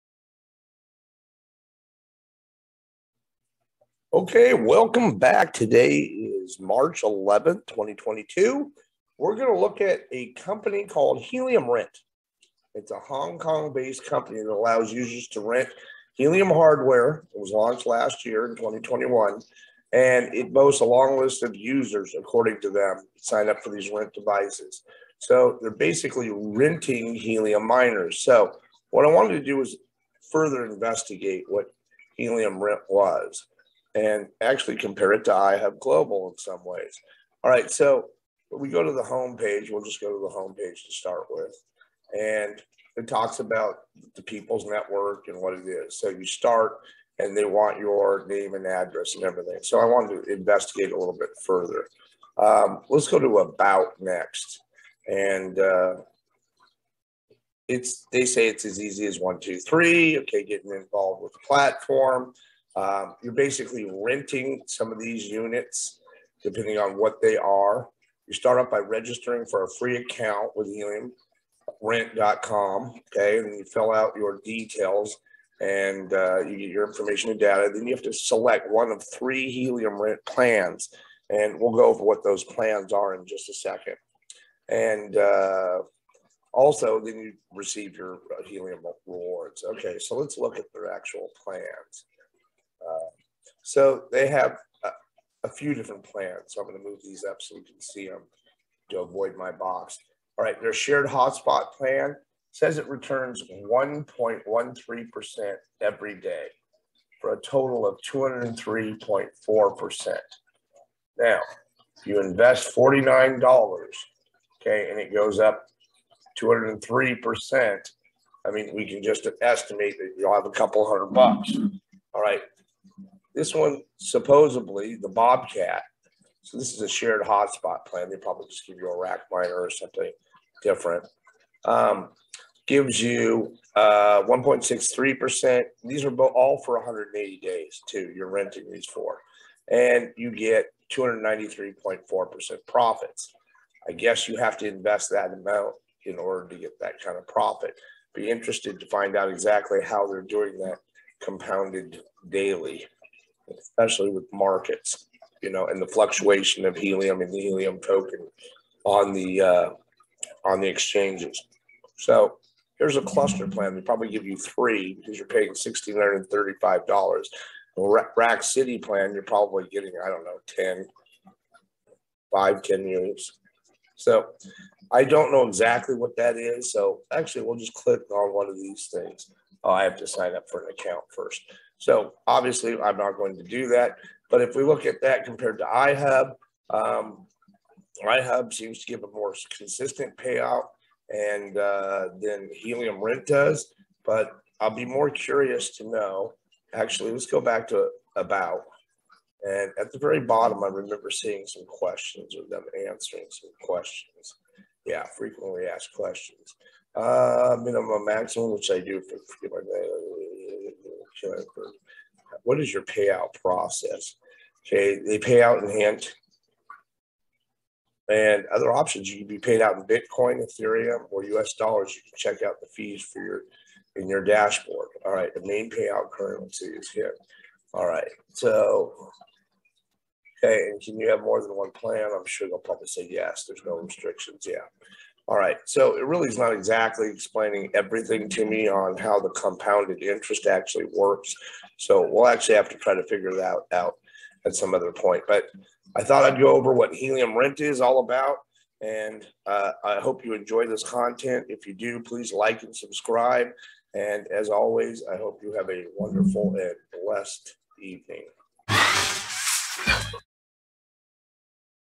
Helium Rent? New Competitor to Ihub?
This week's news that describes the launch of a new Helium Renting Company